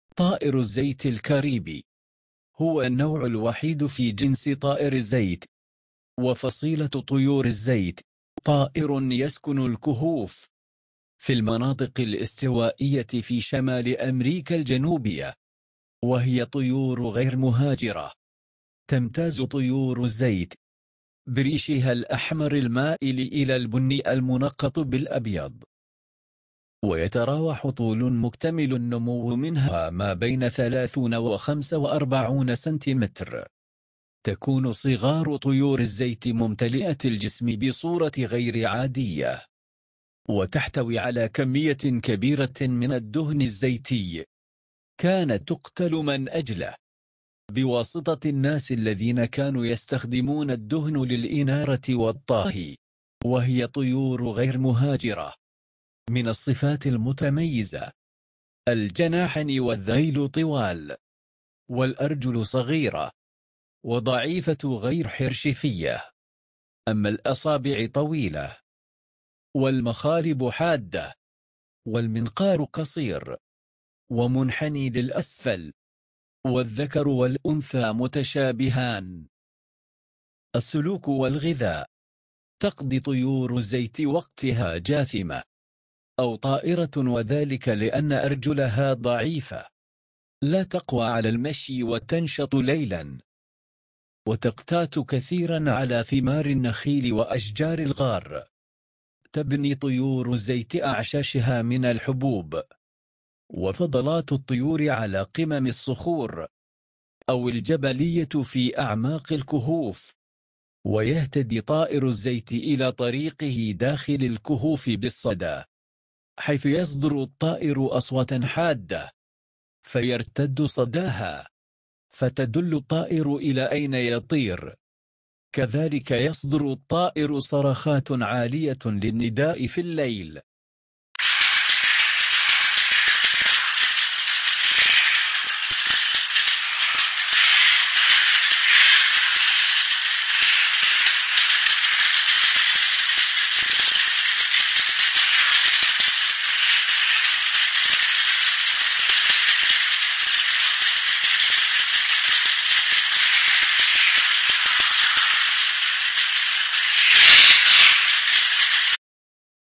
طائر الزيت الكاريبي
الأسم العربي: طائر الزيت
الاسم الإنجليزي : Oilbird
الاسم العلمي : Steatornis caripensis
طائر الزيت الكاريبي  هو النوع الوحيد في جنس طائر الزيت وفصيلة طيور الزيت.
طائر الزّيت طائر يسكن الكهوف، في المناطق الاستوائية في شمال أمريكا الجنوبية. وهي طيور غير مهاجرة.

الوصف
تمتاز طيور الزيت بريشها الأحمر المائل إلى البني المنقط بالأبيض، ويتراوح طول المكتمل النمو منها ما بين 30 و45سم. تكون صغار طيور الزيت ممتلئة الجسم بصورة غير عادية وتحتوي على كمية كبيرة من الدهن الزيتي؛ كانت تقتل من أجله بوساطة الناس الذين كانوا يستخدمون الدهن للإنارة والطهي. تسمى طيور الزيت أيضا جواكاروز.
من الصفات المميزة له أن الجناحان والذيل طوال والأرجل صغيرة وضعيفة غير حرشفية. أما الأصابع فطويلة والمخالب حادة والمنقار قصير وقوي ومنحن للأسفل ومعقوف فتحته طويلة منتصبة.   والذكر والأنثى متشابهان.

السلوك والغذاء
تقضي طيور الزيت وقتها جاثمة أو طائرة وذلك لأن أرجلها ضعيفة لا تقوى على المشي. وتنشط ليلاً وتقتات كثيرًا ثمار النخيل وأشجار الغار.تبني طيور الزيت أعشاشها من الحبوب وفضلات الطيور على قمم الصخور أو النتوءات الجبلية في أعماق الكهوف. ويهتدي طائر الزيت إلى طريقه داخل الكهوف بالصدى حيث يصدر الطائر أصواتًا حادة فيرتد صداها فتدل الطائر إلى أين يطير. كذلك يصدر الطائر صرخات عالية للنداء في الليل
Bengali: ি঄ময়রফাডি
Bengali (transliteration): aẏēlabārḍa
Catalan: Guácharo, Ocell de l'oli
Czech: Gvacaro jeskynní, gvačaro jeskynní
Danish: Fedtfugl
German: Fettschwalm
English: Guacharo, Oilbird
English, HBW: Oilbird
English (IOC): Oilbird
English (United States): Oilbird
Esperanto: Guaĉaro, Gvaĉaro, Oleobirdo
Spanish: Guacharo, Guácharo, Huira pishco
Spanish (Chile): Guácharo
Spanish (Colombia): Guácharo
Spanish (Costa Rica): Guacharo, Guácharo
Spanish (Ecuador): Guácharo, Huira pishco
Spanish (Spain): Guácharo
Spanish (HBW): Oilbird
Spanish (Panama): Guácharo
Spanish (Peru): Guácharo
Spanish (Venezuela): Guácharo
Estonian: õlilind (guahaaro)
Finnish: Rasvakehrääjä
French: Diablotin, Guacharo de Caripe, Guacharo des cavernes
Croatian: uljašica
Hungarian: zsírfecske
Icelandic: Spikfugl
Italian: Guaciaro
Japanese (romaji): aburayotaka
Japanese: アブラヨタカ
Kazakh: гуахаро
Kirghiz: гуахаро, семиз байкуш
Scientific: Steatornis caripennis, Steatornis caripensis, Steatornis carpensis
Lithuanian: Guacharai, Guacharas
Dutch: Vetvogel
Norwegian Nynorsk: Feittfugl
Norwegian: Fettfugl
Polish: tluszczak, tłuszczak
Portuguese: guácharo
Portuguese (Brazil): guacharo, Guácharo
Portuguese (Portugal): Guácharo
Russian: Гуахаро, Жиряк
Slovak: guačaro jaskynný, Guacharo jaskynný
Serbian: Uljana ptica (guačaro)
Swedish: Oljefågel
Turkish: Yağkuşu
Ukrainian: Гуахаро
Uzbek: гуахаро, жиряк
Chinese: 油鸱
Chinese (Traditional): 油鴟